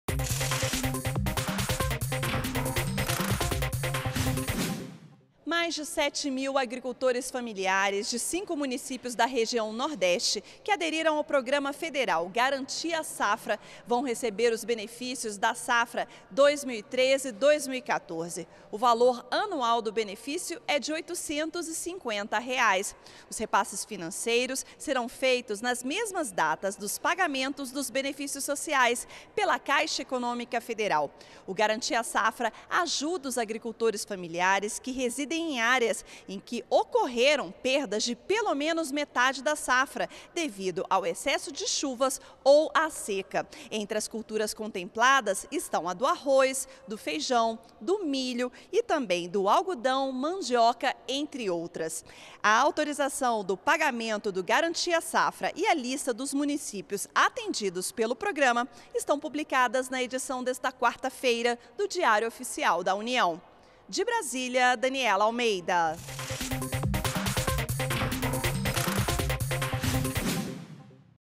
Garantia Safra beneficiará mais de 7 mil agricultores familiares
ÚLTIMAS - 17.06.15: Cerca de 7 mil agricultores familiares, de cinco municípios do nordeste, receberão os benefícios da safra 2013/2014 pelo programa Garantia Safra. A ação do governo federal auxilia pequenos agricultores que sofreram perdas de pelo menos metade da produção, devido ao excesso de chuvas ou à seca. A autorização do pagamento do Garantia Safra e a lista dos municípios atendidos podem ser acessadas na página do Diário Oficial da União